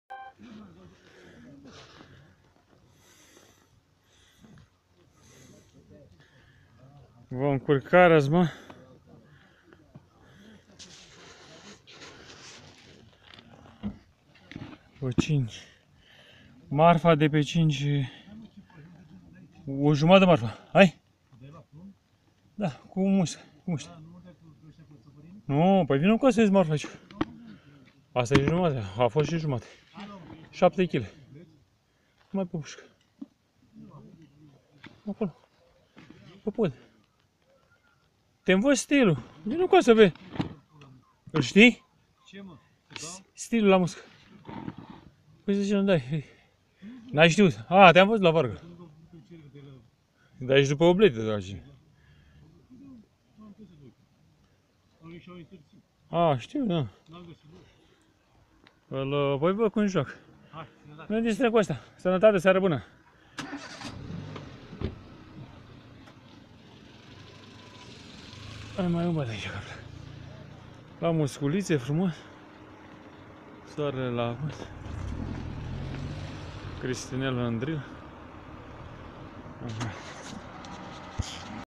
PESCUIT BABUSCA LA BARAJUL BUDEASA 5-03-2016
BABUSCA AM PESCUIT-O LA BARAJUL BUDEASA PE VERTICALA,INTRE APE CU PLUMB SI MUSTE ARTIFICIALE PE FIR.PENTRU COMENZI DE MUSTE ARTIFICIALE FOLOSITI NUMARUL MEU DE CONTACT:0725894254,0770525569.